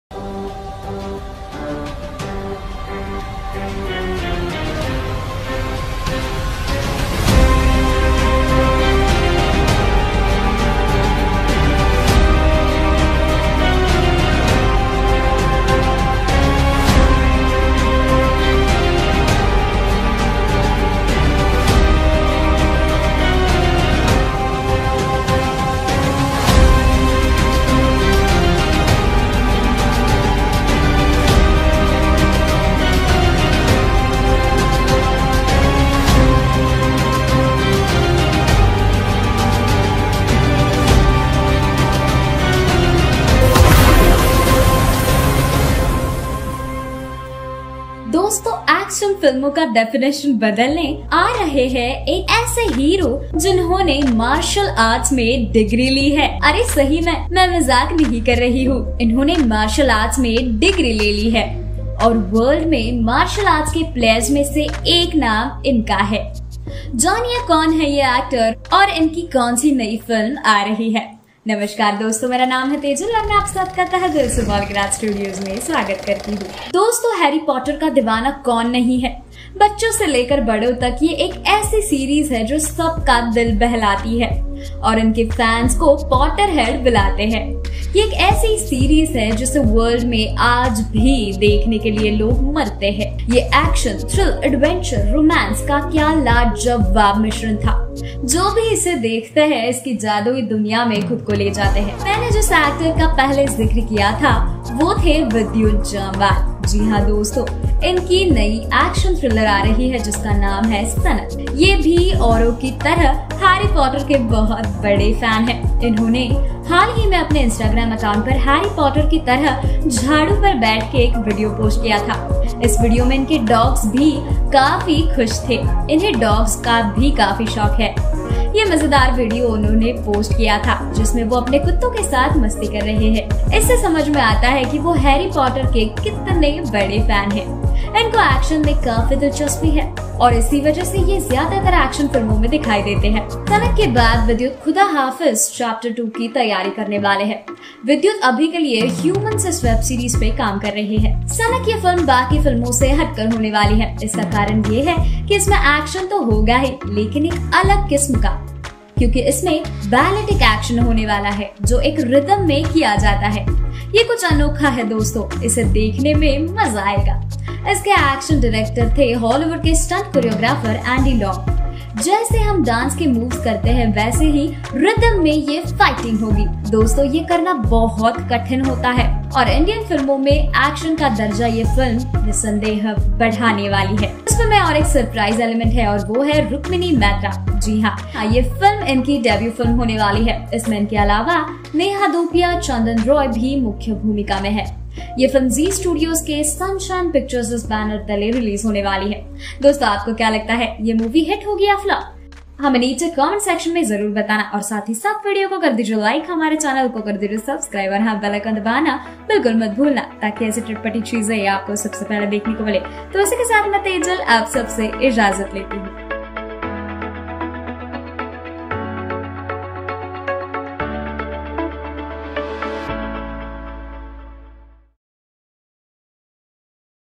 Sanak | 21 Interesting Facts | Vidyut jammwal | Rukmini maita | Concept trailer | Neha chupia
0:00 Introduction ?
1:06Vidyut jammwal to play lead role in sanak ?
2:01 Is vidyut jammwal fan of harry potter?
3:00 actress in upcoming movie sanak  ?
Guys check out Sanak movie top interesting facts by bollygrad studioz. Sanak  is an upcoming Hindi movie. The movie is directed by Kanishk Varma and will feature Vidyut Jammwal and Rukmini Maitra as lead characters

This video deals with the comparison conflict of the upcoming action thriller movie sanak, with the already released hit film franchise commando. as both commando and Sanak starres Vidyut Jamwal in the lead role and an action hero like him is is set to act in another action thriller, this made the viewers curious about what he can deliver more in this action or will it be identical with commando movie series or not.

It will be a really exciting experience to watch the pairing of Vidyut and Rukmini in the film Sanak. It is also easy to connect to the story by seeing fresh faces. But with this, today let us tell you how was the work experience of these two lead actors. Let me tell you Vidyut Jamwal's name is like that work i.e., electrifying every performance. While boys are fans of his action, girls are fascinated by his looks. It was a lot of fun for Vidyut due to the combination of action and emotion in this film, as well as he got along with his co-star. Rukmini Maitra is also very happy and impressed with her costar. Rukmini says that Vidyut is a Polite human being attached to the ground and because of this it was very comfortable to work.
How Rukmini got this role, let me tell you that the team was looking for a new face and Maitra's work in the film Switzerland almost completed the search for the Sunak team. Rukmini was then asked to send the audition video and the rest is history. The film becomes perfect, so Rukmini got selected. Who does not dream of working in Bollywood, but with this, Rukmini believes that the content of the film matters more to her than the language of the film.

Vidyut Jammwal announced his next film, Sanak - Hope Under Seige, Directed by Kanishk Verma, the film also stars Rukmini Maitra.

Produced by Vipul Shah, the film is directed by Kanishk Verma. Chandan Roy Sanyal and Neha Dhupia will be seen in pivotal roles in the film. The makers have been tight-lipped about the films characters. Sanak will be an emotional action-thriller film.

Credits
Script-Tejal Patil
Anchor- Tejal Patil
Editor- Rouvya.